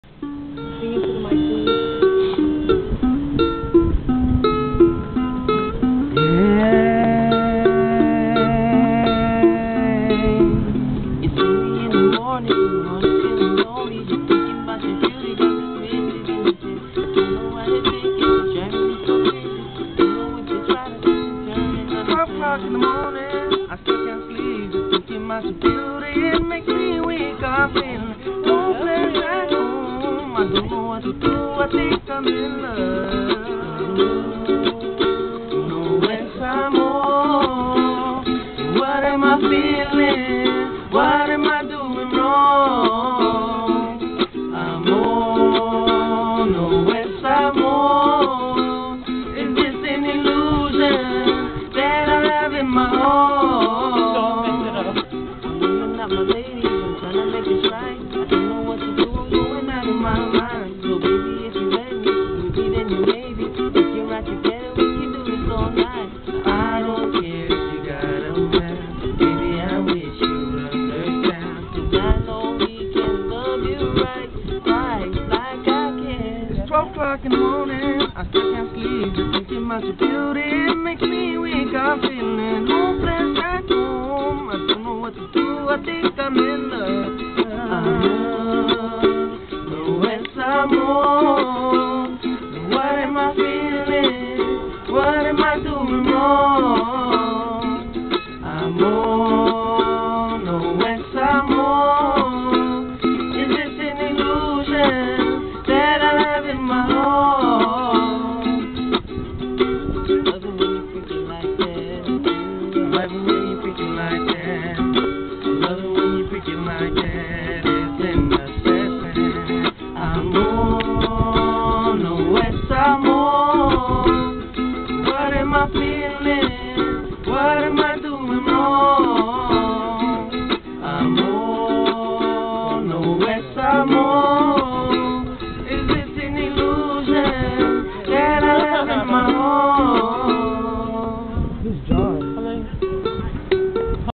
The Twins from Aunu'u
Angel and Alex doing their thang on the pier in Oceanside California!